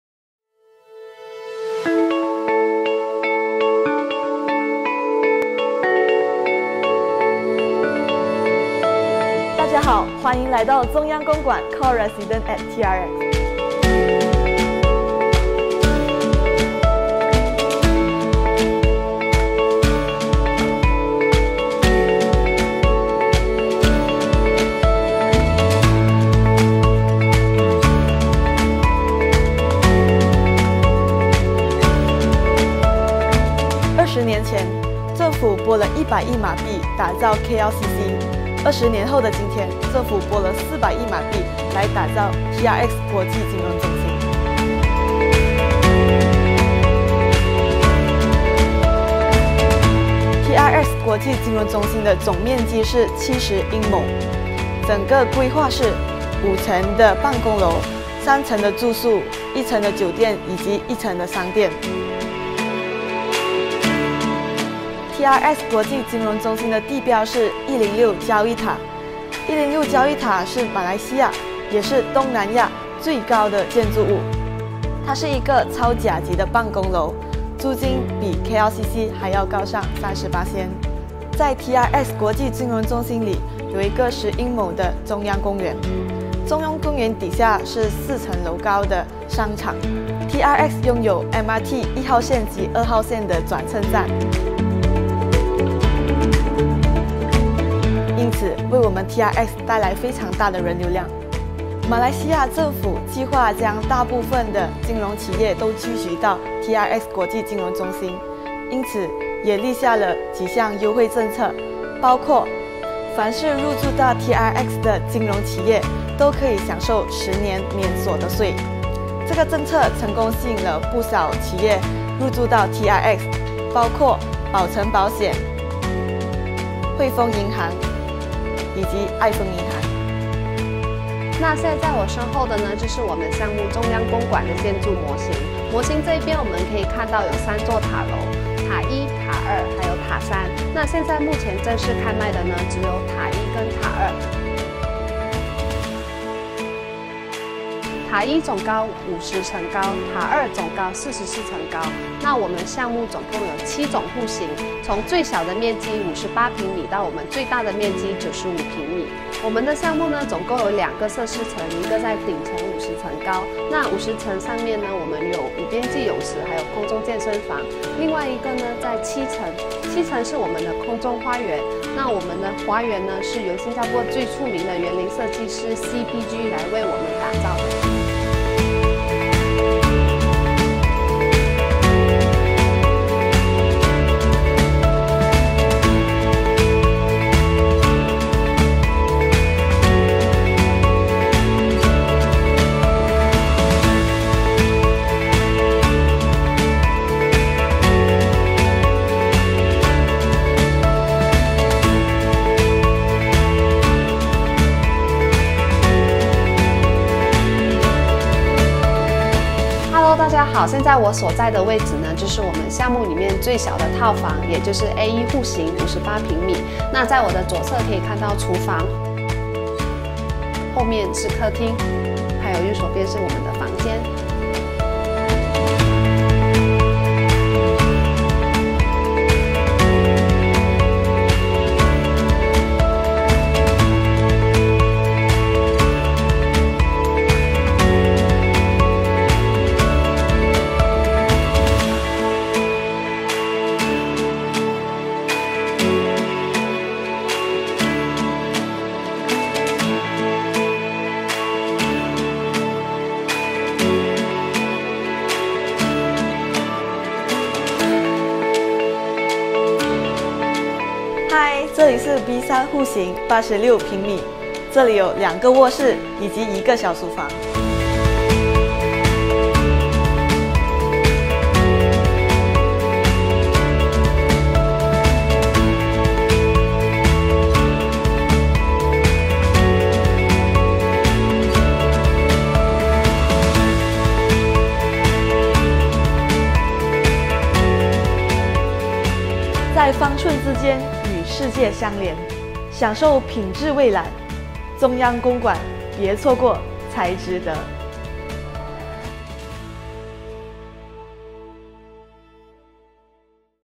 Core Residence @ TRX (Tun Razak Exchange)
Core Residence @ TRX — First Business Suites Apartment in TRX

RM 1.4 billion high-end freehold project will be developed on a 1.65 acre land in Kuala Lumpur International Financial District.

TRX is expected to boost its Gross Development Value (GDV) to more than the estimated RM40 billion with its burgeoning potential as the next mega catalyst development that will spur economic growth and take urban regeneration to the next level.

Geared towards gathering over 250 top-tier international financial companies, TRX is set to create 500,000 jobs and 40,000 knowledge workers specifically for financial services. International institutions, government agencies and related enterprises are also set to make TRX their bases of operations.